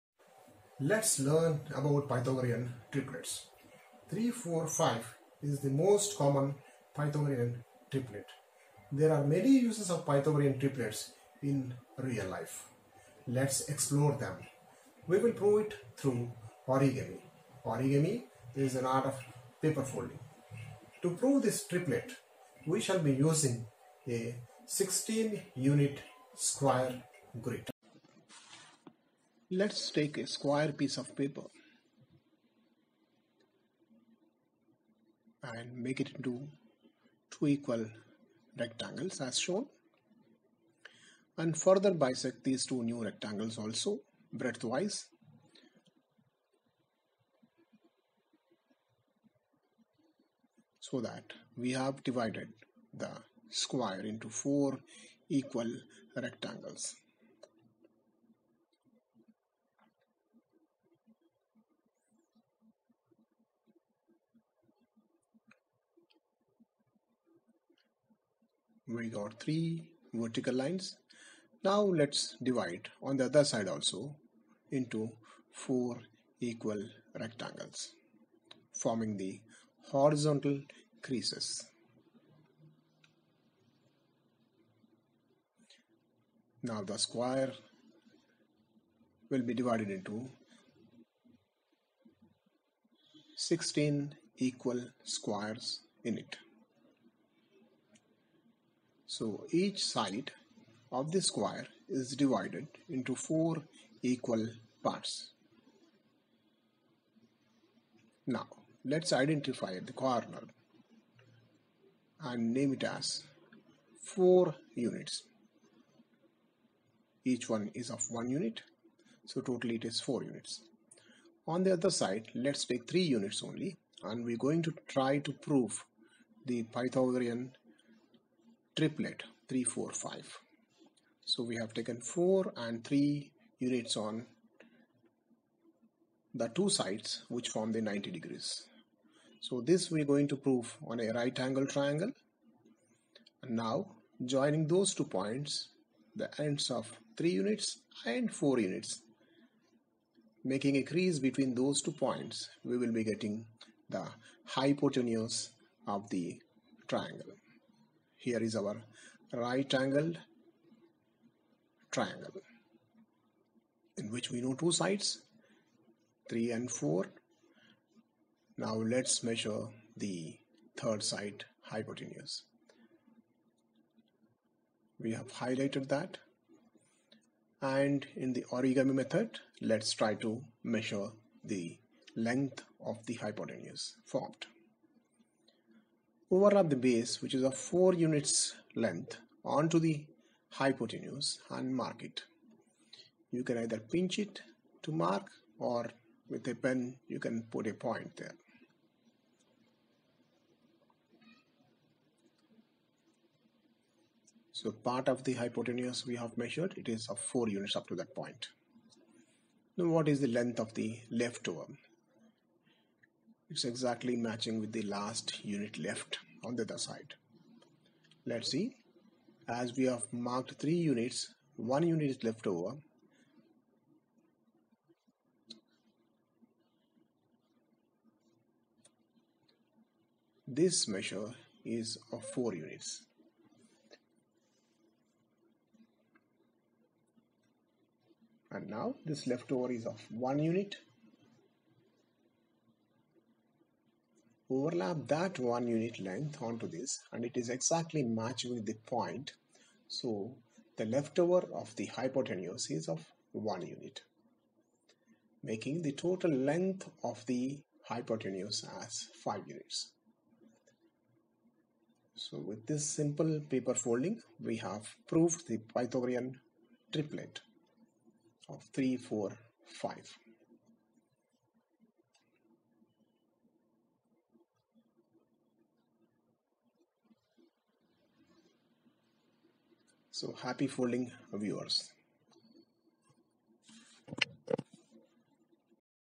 Origami Pythagorean triplet 3,4,5 - Ravi kumar Toleti
We can prove pythagorean triplets using origami. To get the concept of the triplets this video is done. And to understand more about Pythagoras theorem see the video of this channel